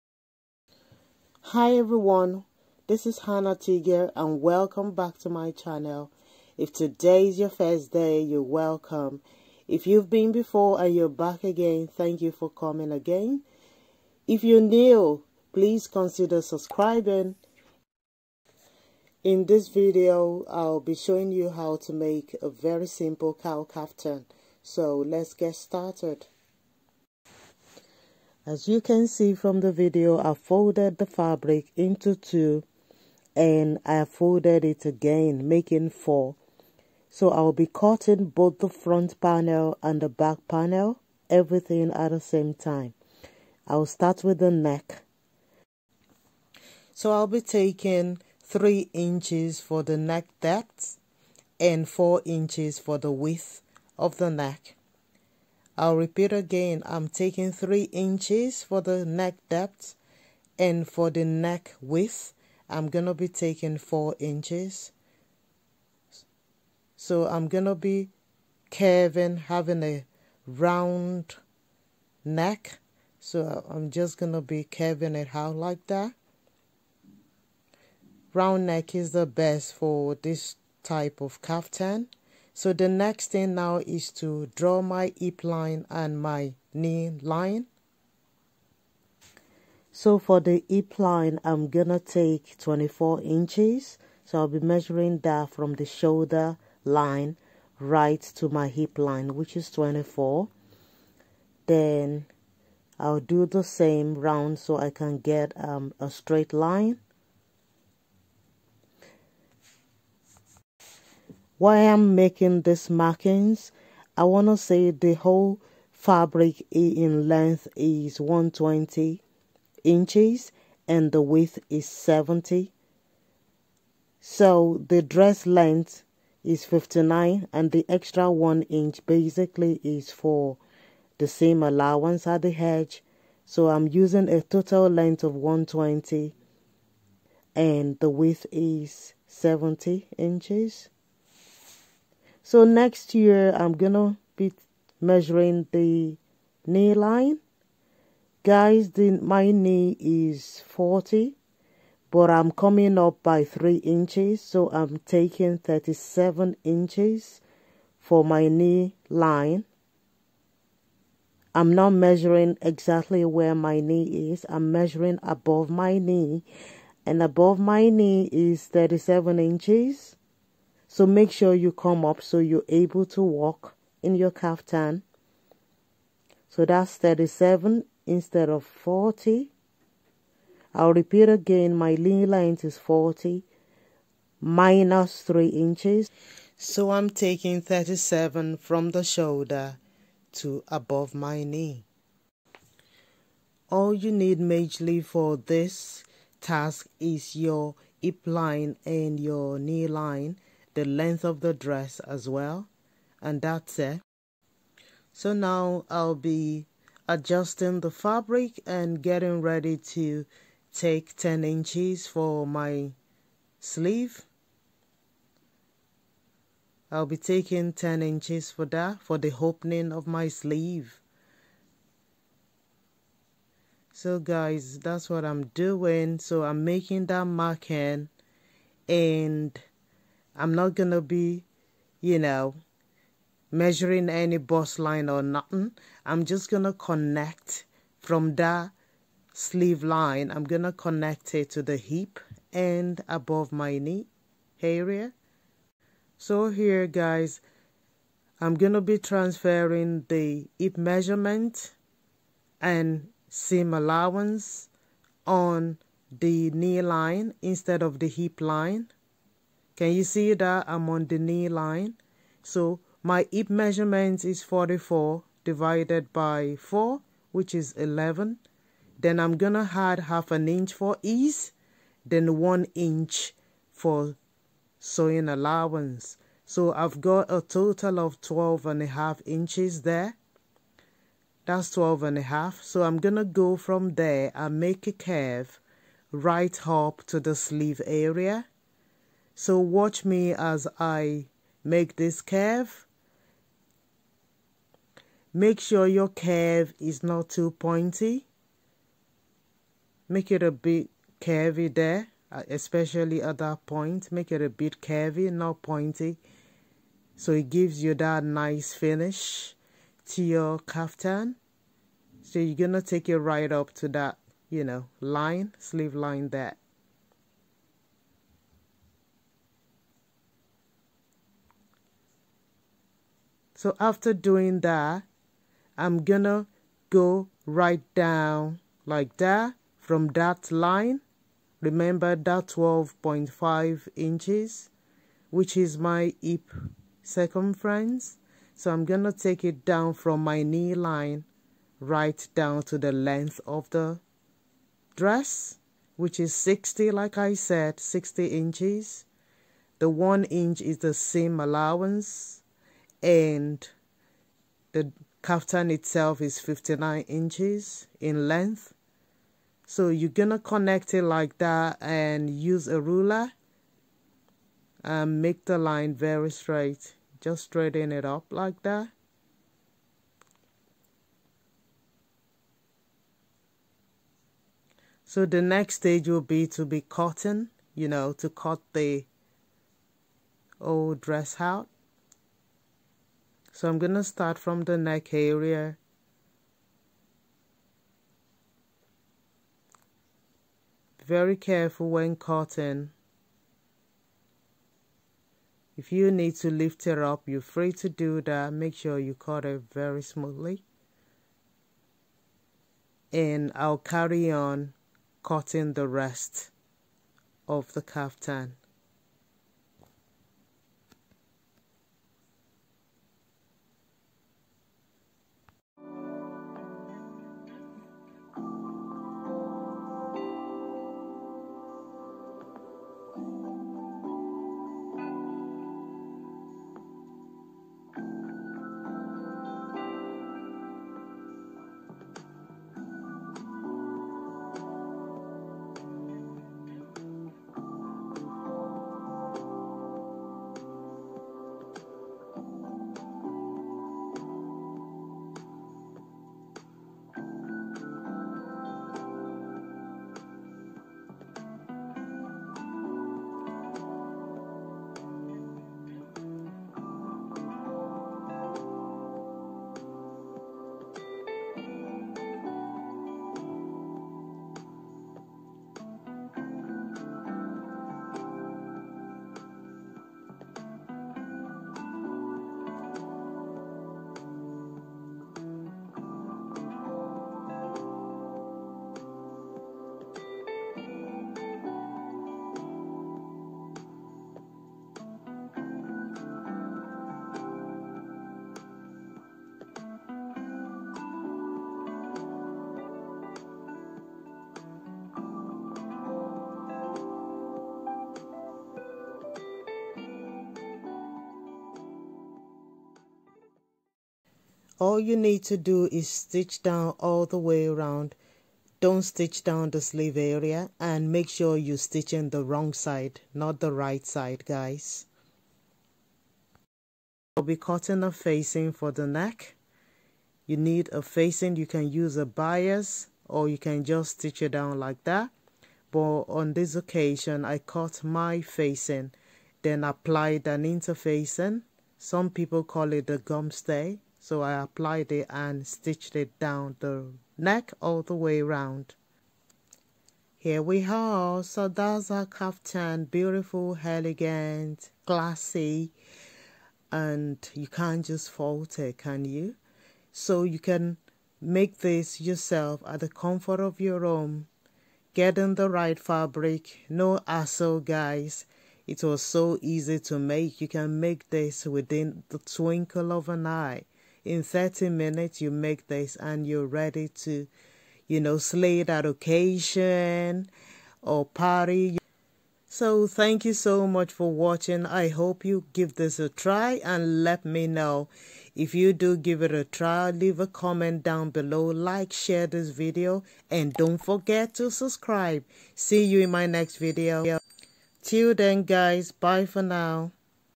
HOW TO MAKE A COWL KAFTAN || NO PATTERN DIY
Easy cowl kaftan in 30 minutes with detailed instruction. Very classy and trendy occasion apparel. You will enjoy making this.
You need 2 metres and a quarter fabric if your height is 57- 59 inches.
The width of the fabric is :70 inches.
Some fabric do not have that much in width. Normally between 45 - 60 inches.
Fabric used is Rayon Spandex Fabric

💥WATCH OTHER TUTORIALS